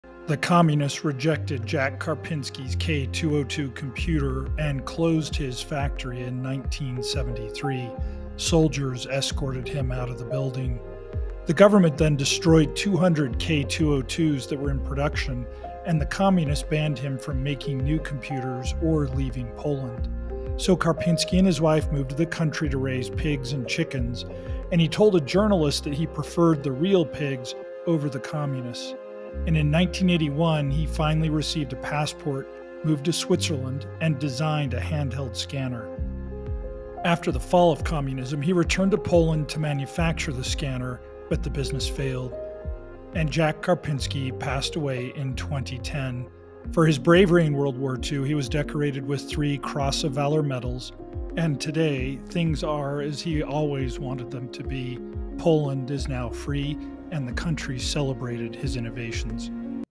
How a Communist Goverment Tried & Failed to Shut Down Jacek Karpiński
The communists couldn’t accept Jacek Karpinski’s K-202 computer and closed his factory in 1973.  Soldiers escorted him out.  The government destroyed 200 K-202s that were in production.  The communists banned him from making computers or leaving Poland.  Karpiński and his wife moved to the country to raise pigs and chickens. He told a journalist that he preferred the real pigs over the communists. In 1981, he finally received a passport, moved to Switzerland and designed a hand-held scanner.  After the fall of communism, he returned to Poland to manufacture the scanner, but the business failed.  He passed away in 2010.  For his bravery in WW2, he was decorated with three Cross of Valour medals. And in 2024, I found a few examples of his innovation in Warsaw’s Museum of Technology, including the KAR-65, AKAT-1 and the K-202.  Things are as he wanted them to be – Poland was free and celebrated his innovation.